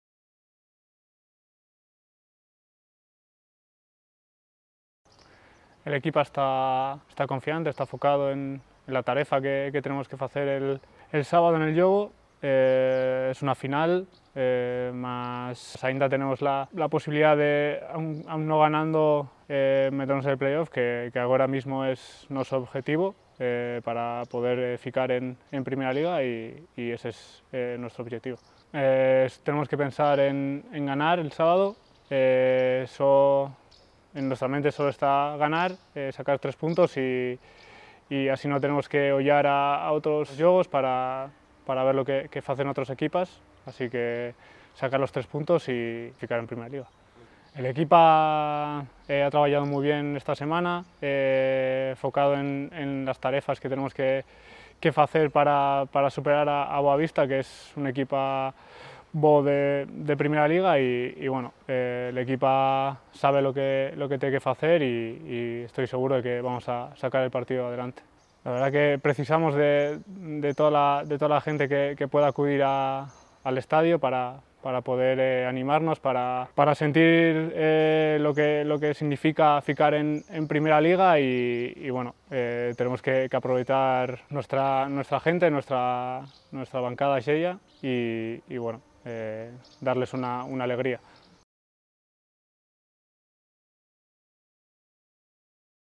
Undabarrena | "Ganhar! É esse o nosso único pensamento"
Sábado há mais uma final e Undabarrena sublinha o único objetivo dos auriverdes 💪